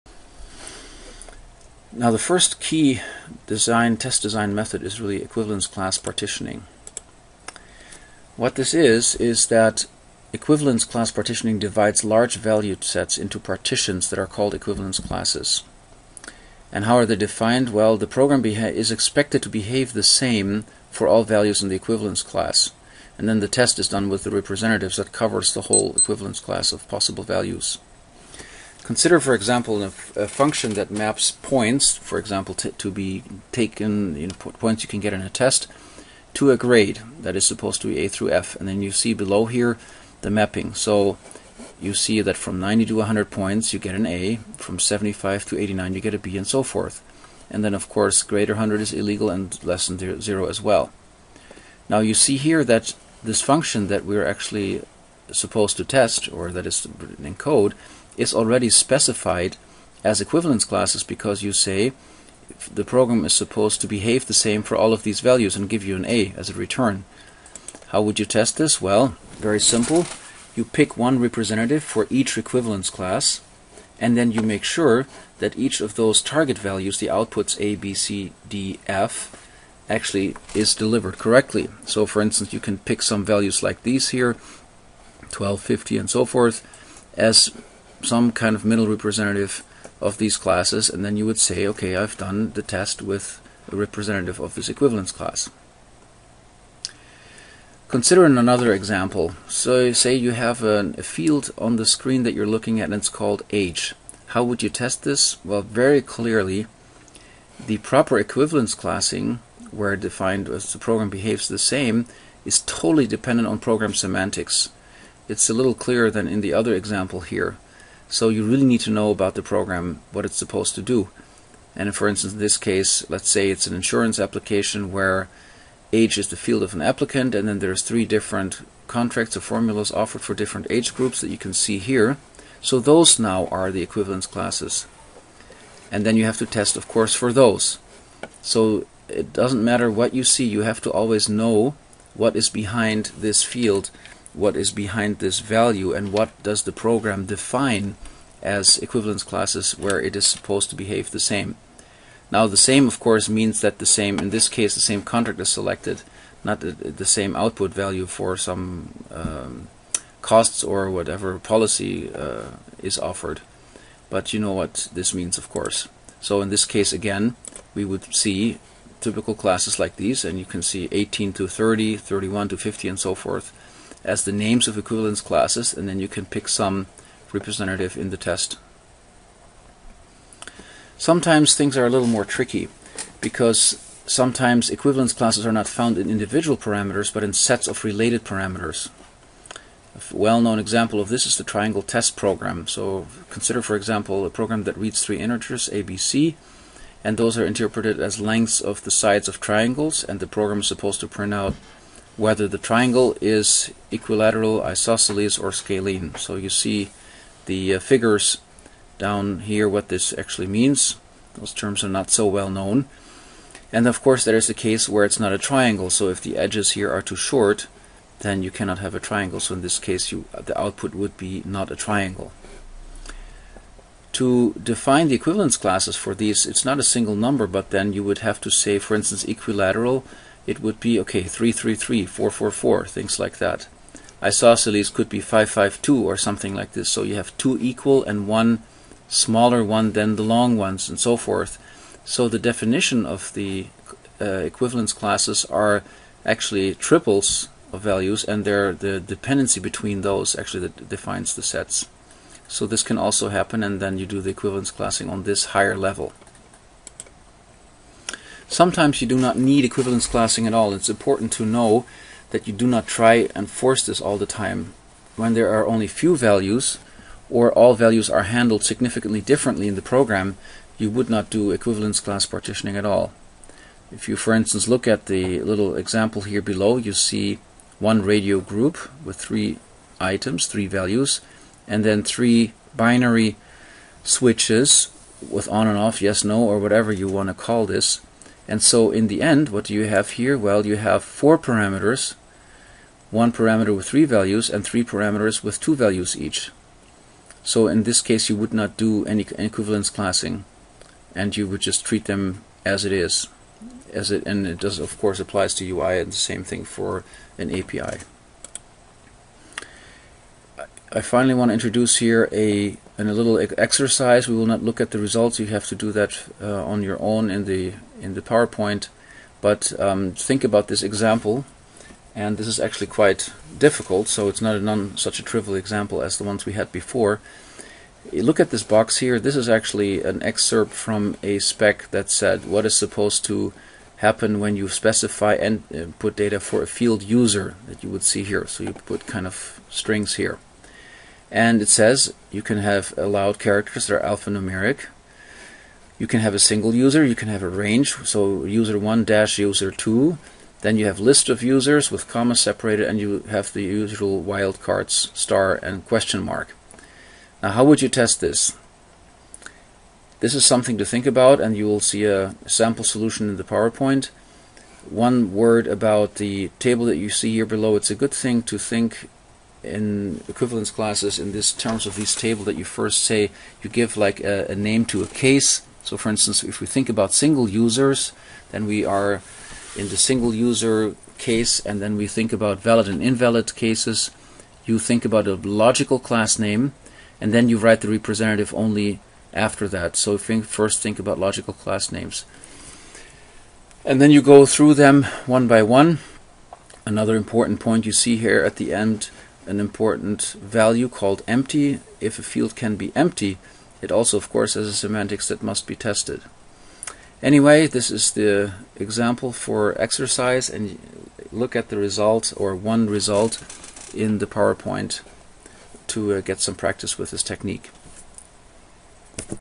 TestDesign 2 - Equivalence Partitioning
Equivalence partitioning identifies sets of input values for which 'the software is expected to behave similar'. You then take a representative of this set and use it for testing.

Presented by Juergen Heymann, SAP, Development Methods